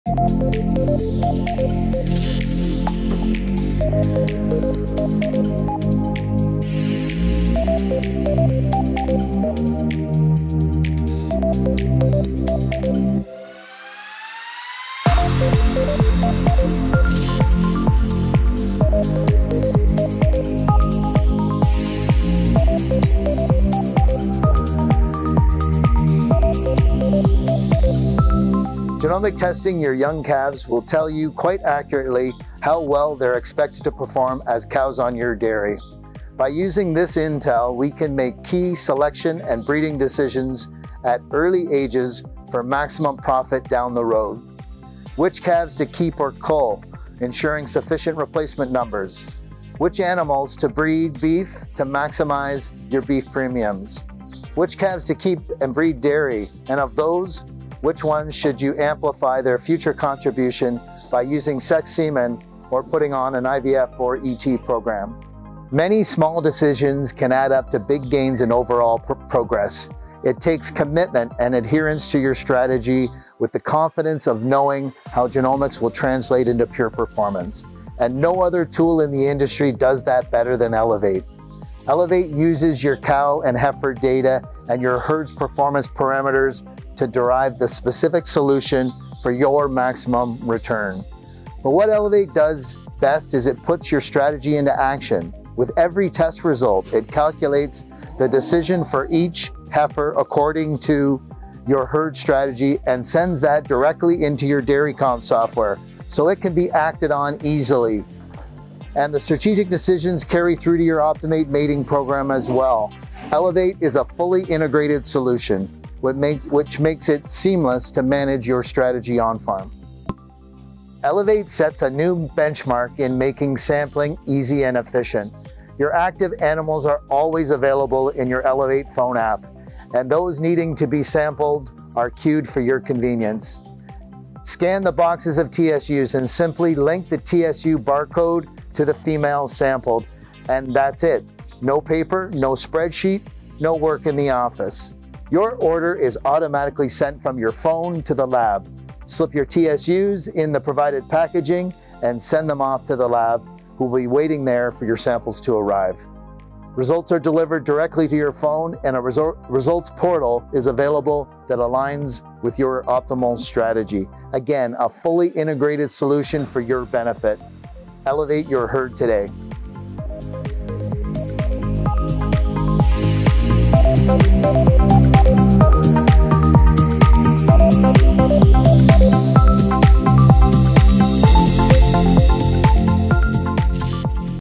Semex ELEVATE® 2022
Semex's Elevate® program gives you fast, easy access to vital female genomic information that increases genetic gain, corrects parentage errors, manages inbreeding and helps you make confident mating decisions and valuable genetic progress.

Elevate identifies your best females through a fast, easy genomic testing program that works with the Semex Solutions app to deliver results right to the palm of your hand. Herd inventory, samples, orders and decisions are made seamlessly without messy spreadsheets or files. Clients tell us it reduces work, limits errors and gives the quick results they need to make fast genetic decisions at an affordable price.

And, an exclusive to Elevate clients, Semex now offers an Immunity genomic test for your females... Immunity genomics and Elevate make your herd the healthiest, most profitable possible!